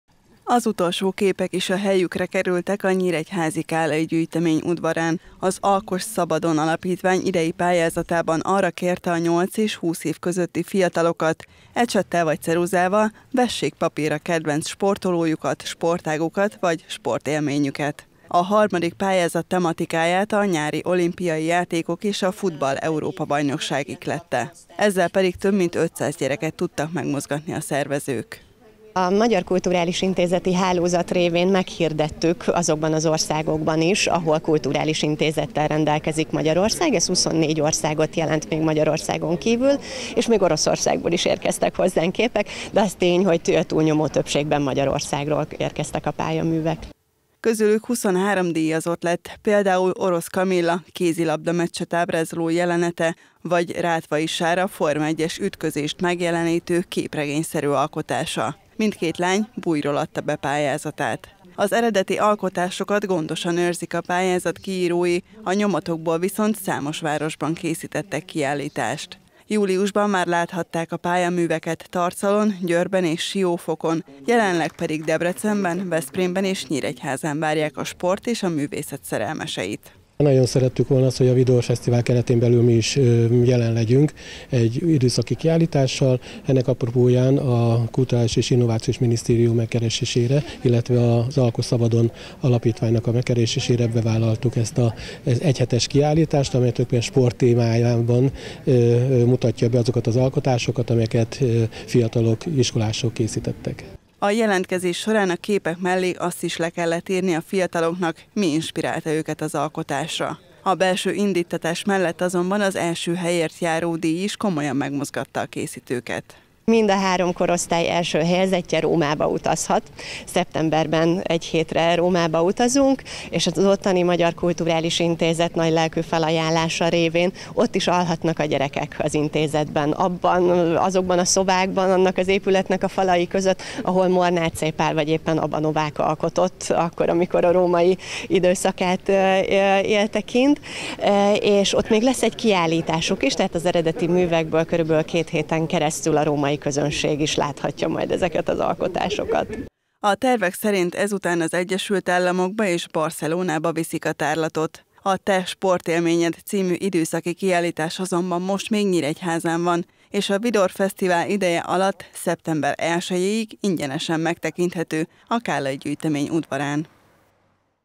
Alkoss szabadon!
Két buji fiatal alkotása és díjazott lett az idei Alkoss Szabadon! pályázat közel 200 alkotása közül. A 23 festményből és rajzból álló kiállítás jelenleg Debrecenben, Veszprémben és Nyíregyházán látható, később Rómába, az Egyesült Államokba és Barcelonába viszik majd, novemberben pedig árverésre bocsátják a műveket.